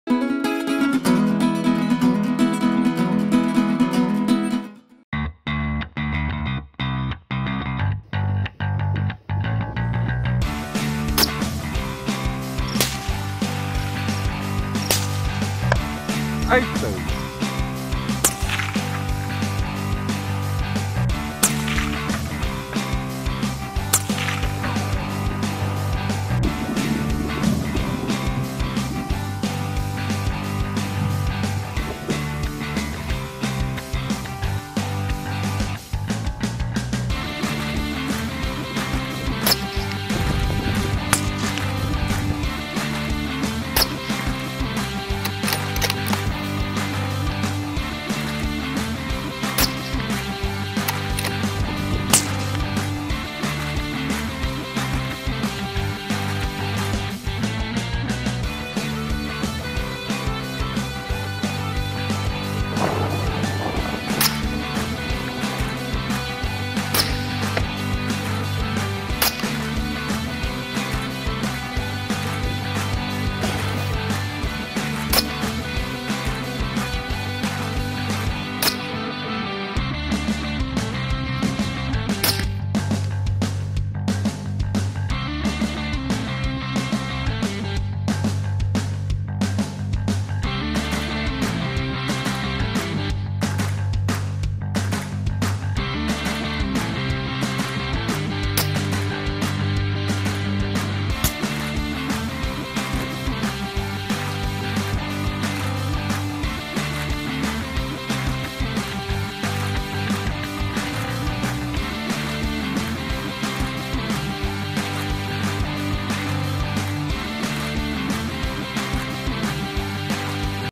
NOT FOR SENSITIVE VIEWERS... Hunting warthog, springbuck and Blesbuck in the Freestate, South Africa
!!!NOT FOR SENSITIVE VIEWERS!!!

Video contains footage of headshots.

Video showing scope cam and normal video footage of hunting Warthog, Springbuck and Blesbuck in the East Freestate area of South Africa with a .243 Howa Ackley Improved caliber, MDT stock, Hawke Sidewinder, Winchester, Hornady bullets 87 gr.